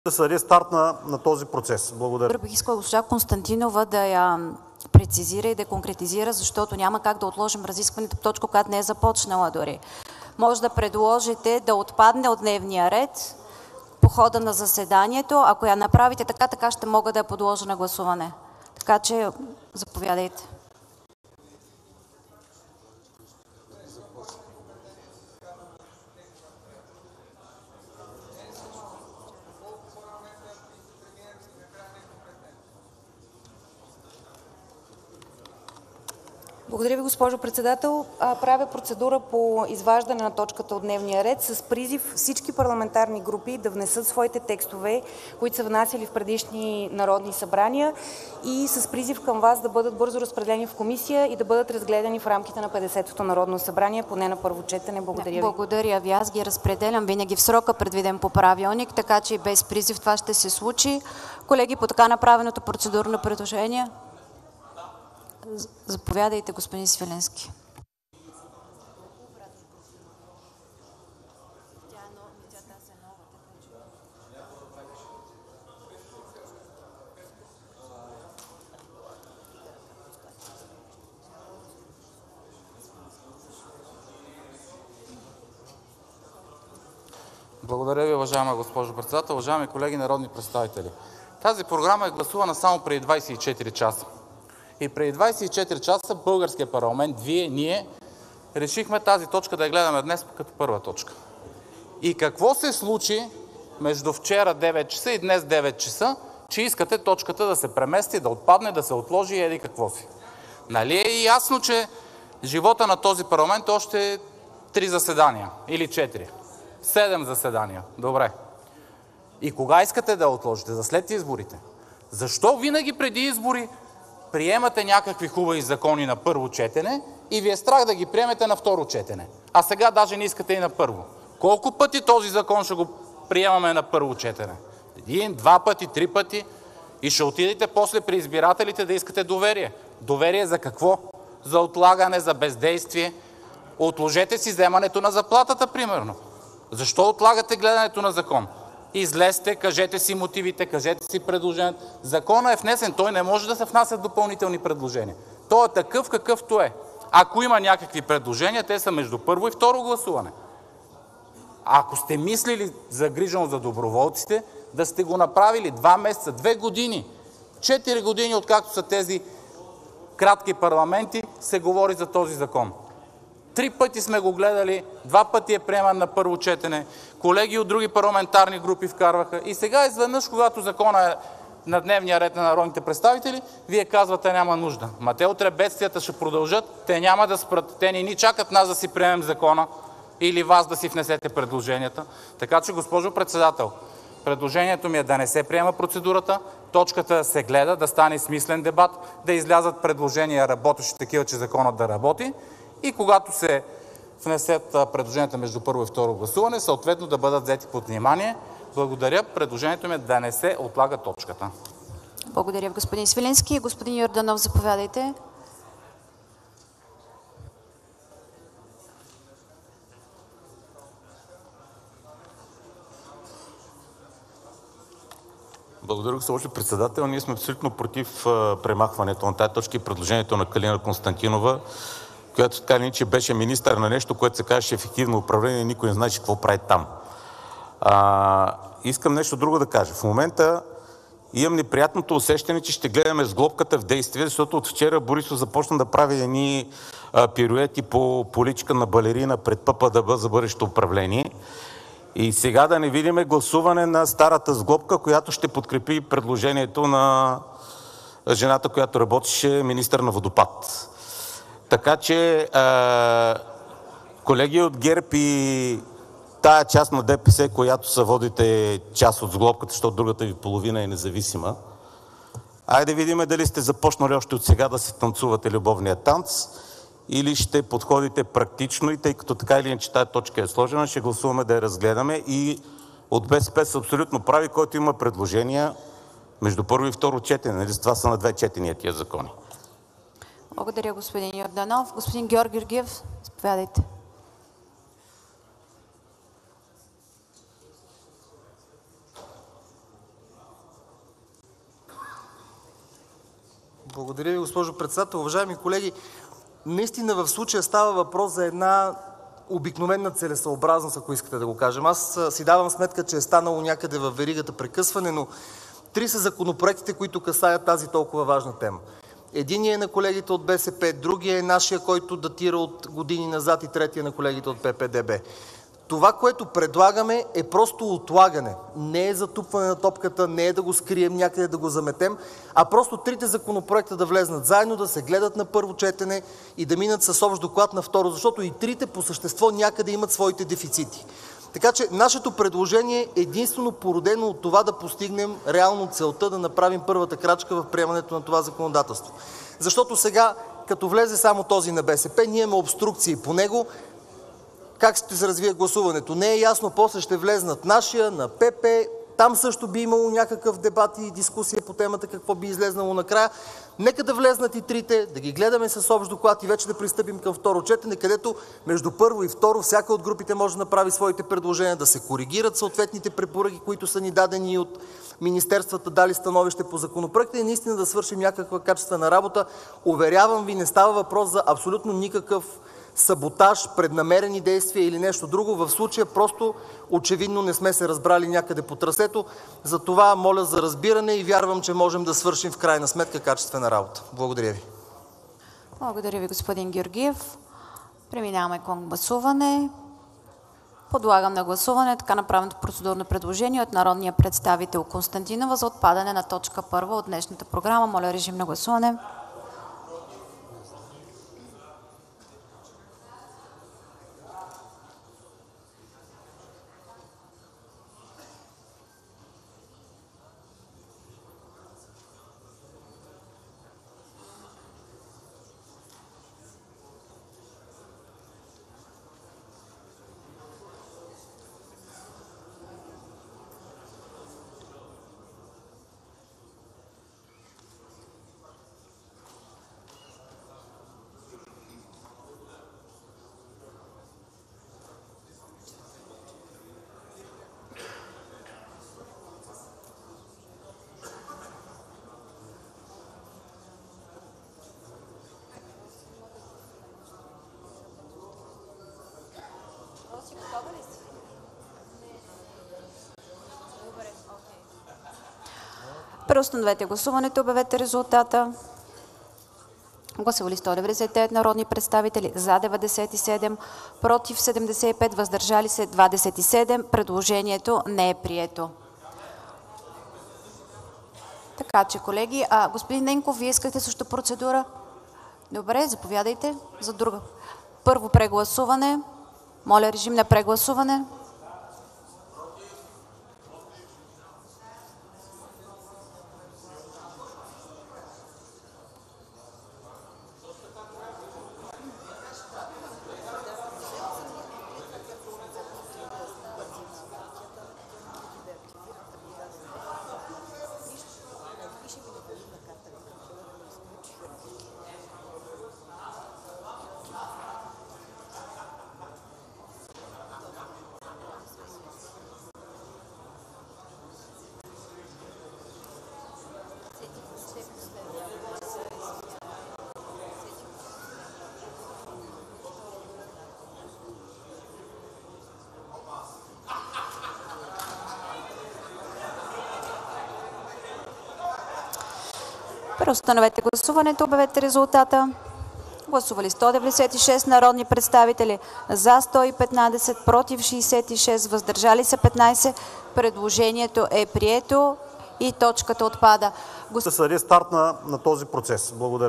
Новата сглобка се тества с неадекватни предложения. Лена или Калина ръководят шоуто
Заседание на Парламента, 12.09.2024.
Гледайте заседанията на Народното събрание  и на комисиите на НС
на живо по телевизия СКАТ+,  както и повторенията от 19 ч.